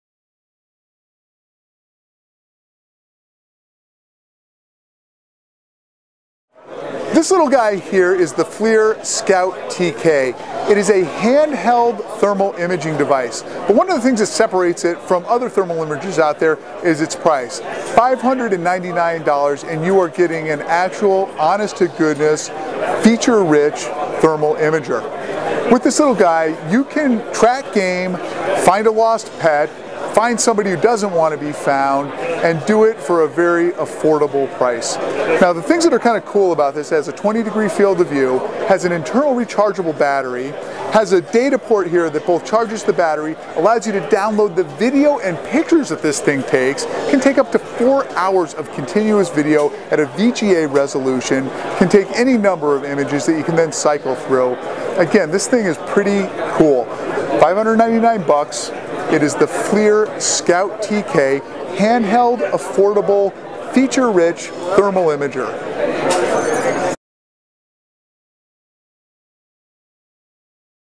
First Look: FLIR Scout TK
John B. Snow checks out this new thermal imaging device.